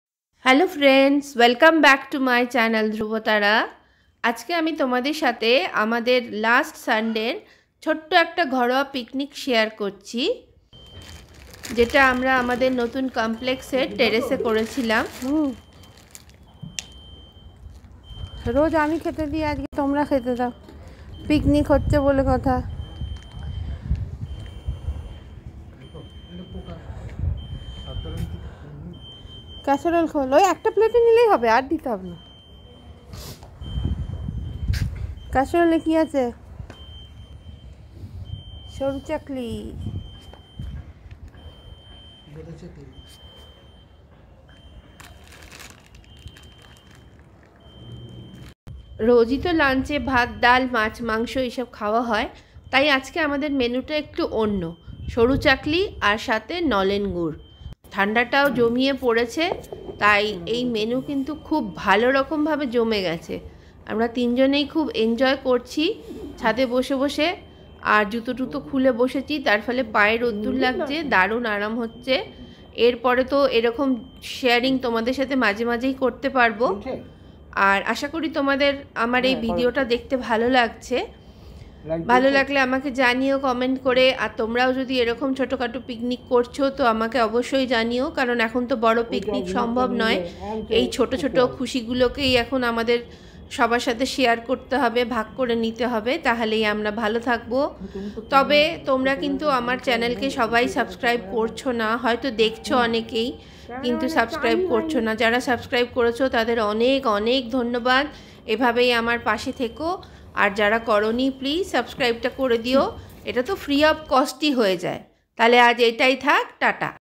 Our Picnic..# family picnic # Sunday enjoyed a Sunday afternoon with family 👪 😊
Today I am sharing with you.

Sunday picnic at our complex. enjoying lunch with family 👪

enjoy  Soruchakli pithe with nolen gur..

watch my many pithe recipes 😋

Be safe and be positive 👍

COMMENTS ✍

TAKE CARE 🙂 ❤